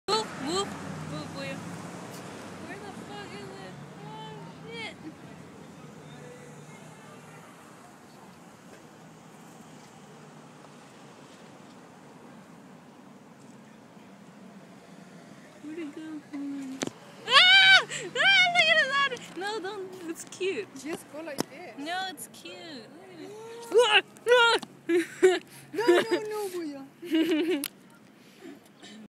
cute black and yellow caterpillar
crazy looking caterpillar i saw while walking the dog.